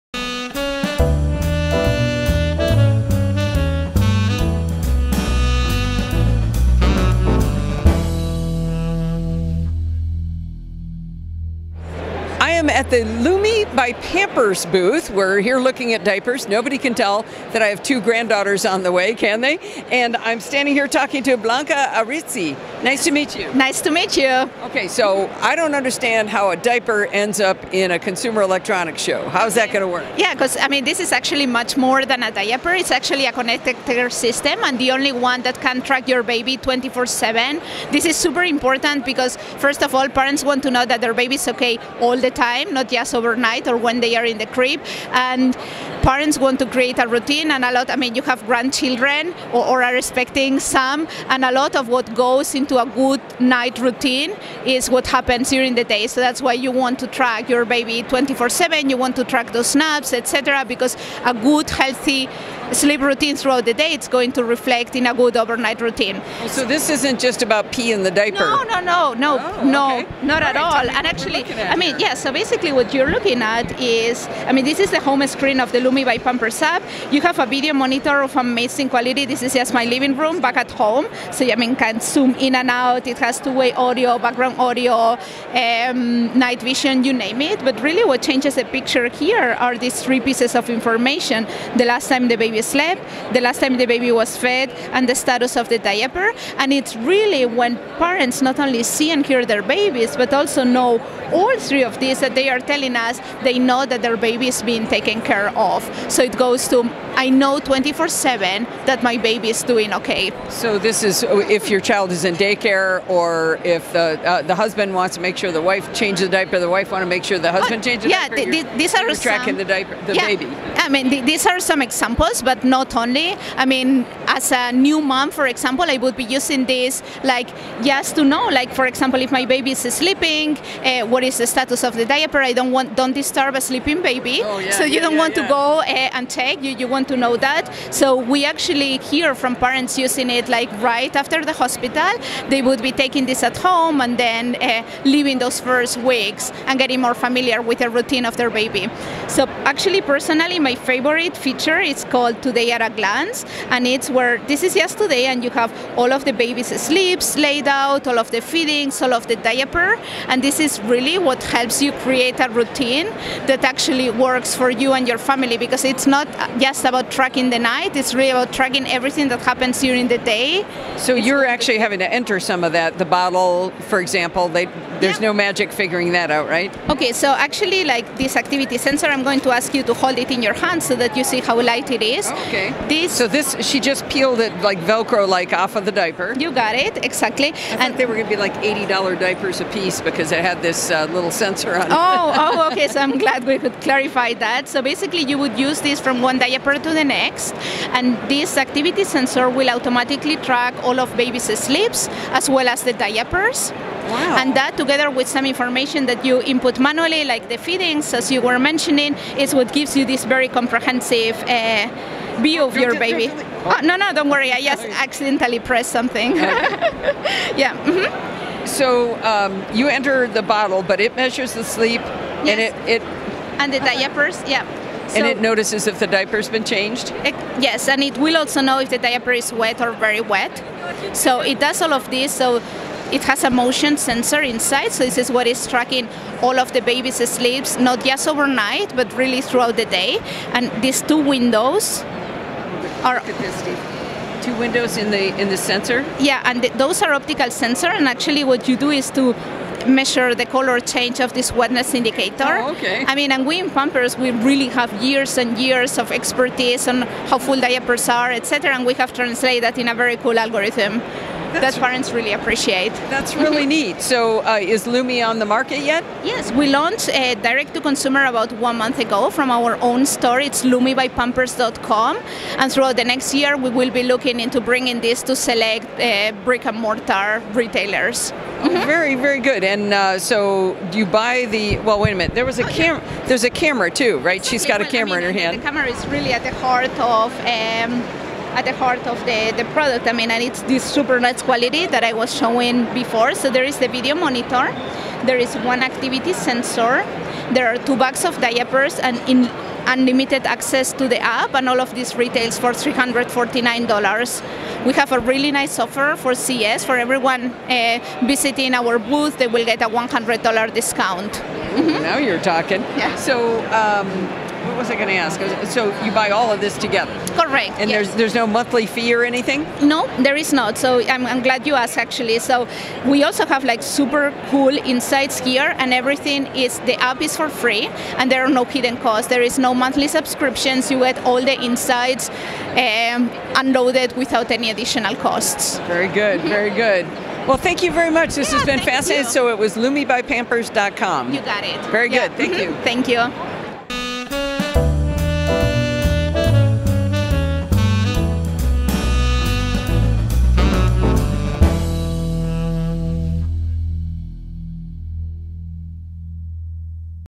CES 2020: Lumi by Pampers
Allison Sheridan interviews Blanca Arizti from Lumi about their new diaper system by Pampers. The Lumi diaper system is a connected system that can track your baby 24/7.  The Lumi diaper has a special pouch that houses an activity sensor which is moved from one diaper to the next. The activity sensor tracks the baby’s sleep and the diaper’s wetness status. The system also comes with a video monitor that has two-way audio and night vision to allow you to monitor your baby. Tying all of this together is the Lumi mobile app that allows you to track when the baby has slept, when the baby has been fed (with user entry), the baby’s diaper status and it provides a real-time video of your baby’s room. The setting is CES Unveiled at the Mandalay Bay Hotel in Las Vegas.